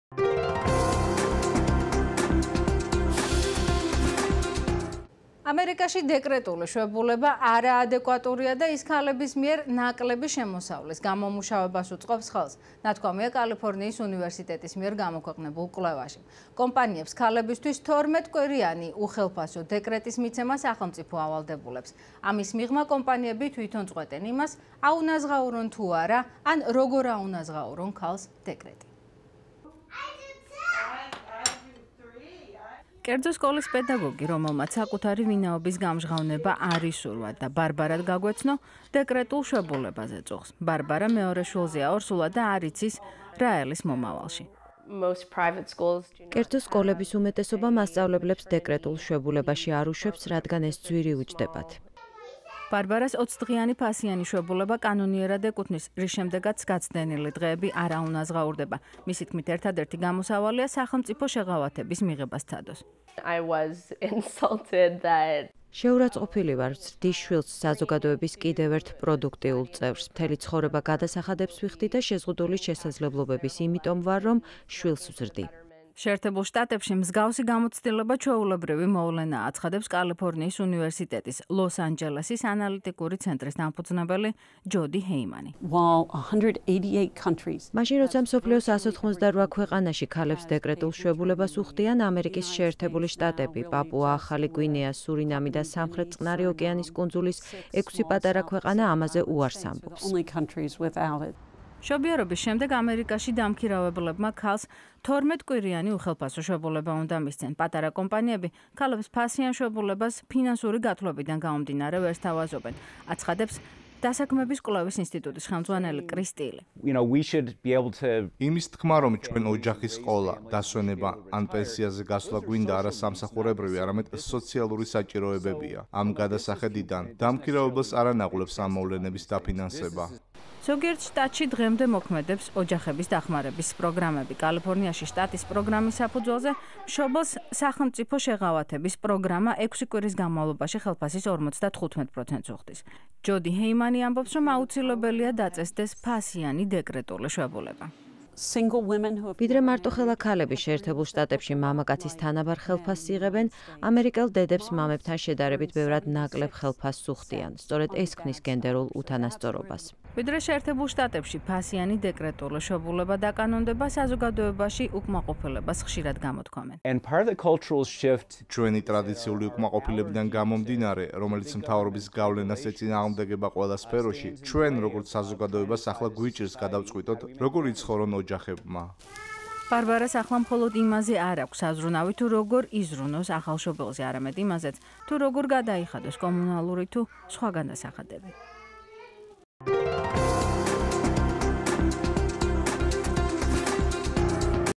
დეკრეტული შვებულება და გენდერული უთანასწორობა
შეერთებულ შტატებში დეკრეტული შვევებულება არაადეკვატურია და ის ქალების მიერ ნაკლები შემოსავლის გამომუშავებას უწყობს ხელს - ნათქვამია კალიფორნიის უნივერსიტეტის მიერ ახლახან გამოქვეყნებულ კვლევაში.